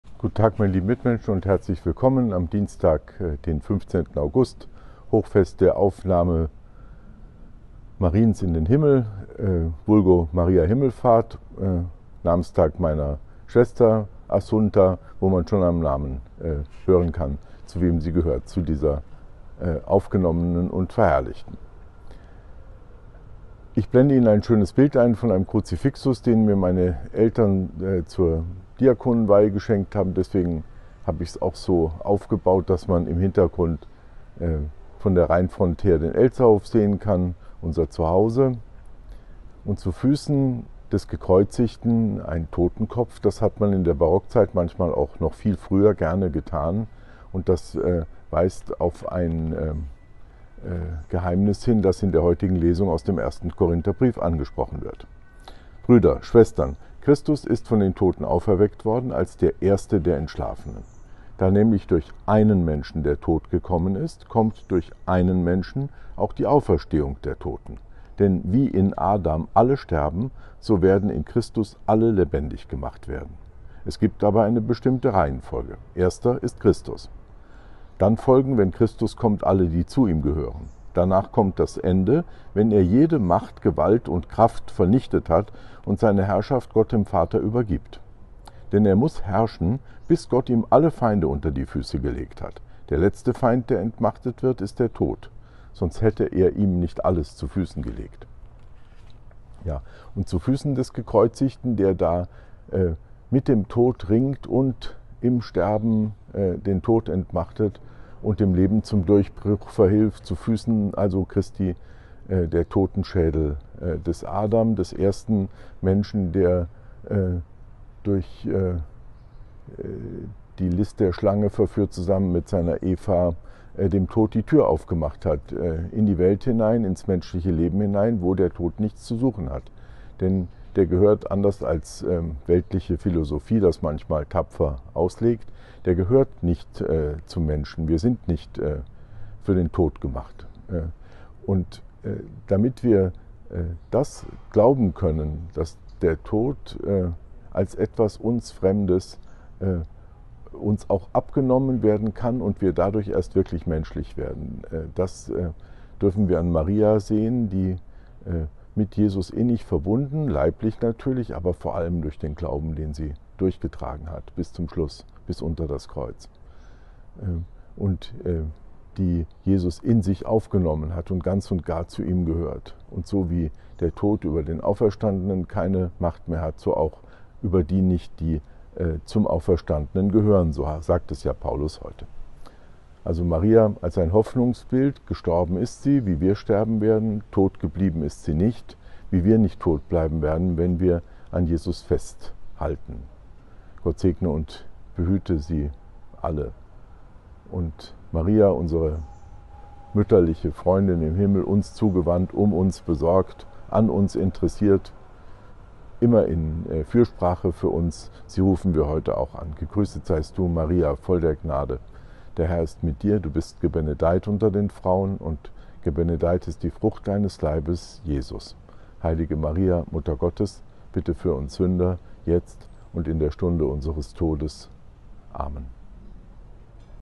Dompfarrei St Bartholomäus Stadtdekan Johannes zu Eltz 15082023
Dienstag, 15. August 2023 – Impuls von Stadtdekan Dr. Johannes zu Eltz, Pfarrer der Dompfarrei St. Bartholomäus in Frankfurt am Main.

Seit Mitte März 2020 finden Sie hier täglich einen geistlichen Impuls der Dompfarrei St. Bartholomäus, mit dem wir Sie durch den Tag begleiten möchten.